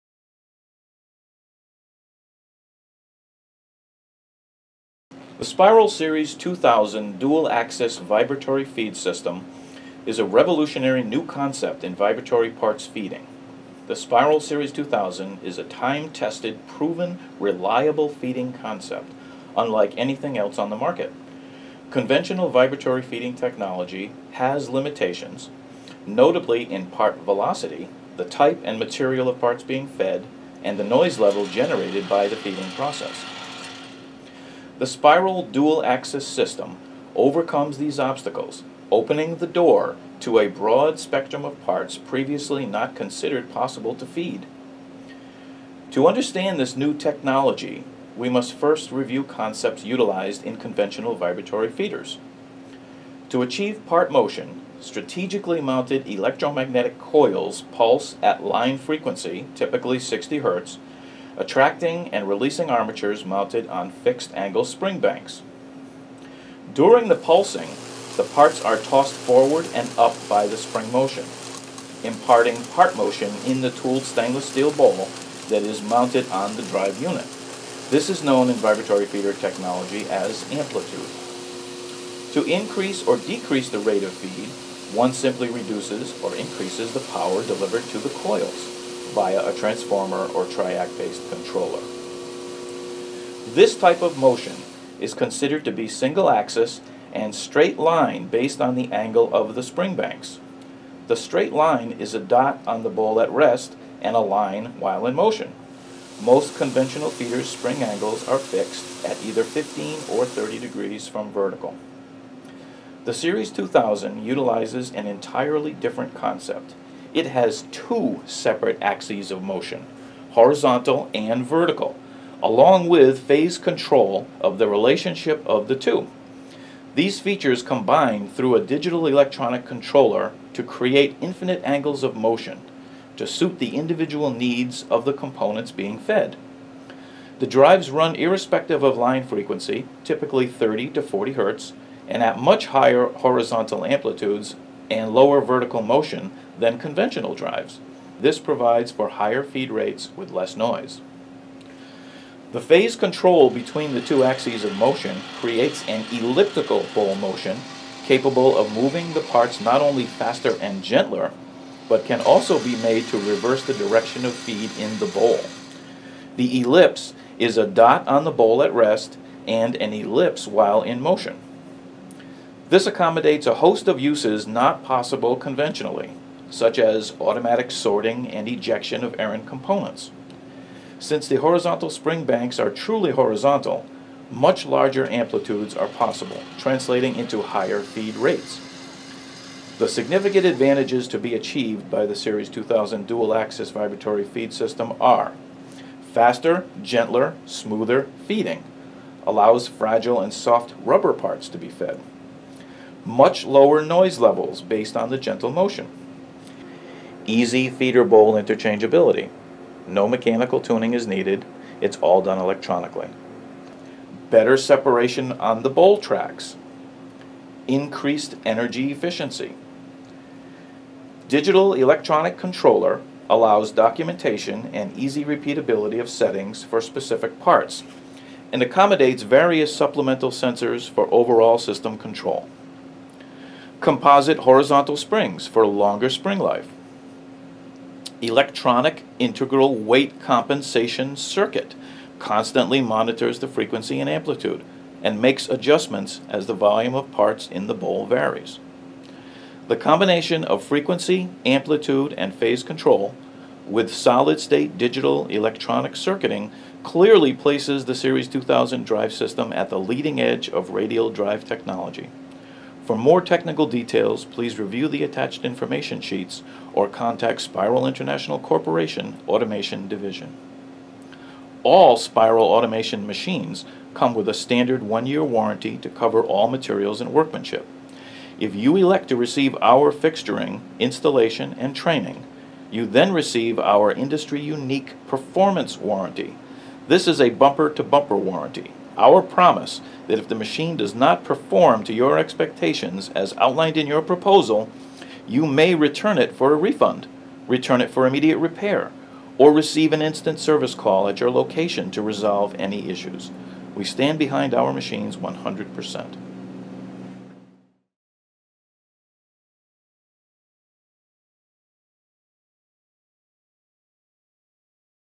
High Performance Vibratory Feed Technology
The leading edge of radial drive technology, SPIROL® Series 2000 High Performance Vibratory Feeders represent the leading edge of drive technology. In automated assembly lines, they offer technology to maximize the efficiency of feed systems and reduce power consumption in the feeding of fuses, tablets, packets, soft rubber parts, fragile parts, and parts that are coated or have a film on them.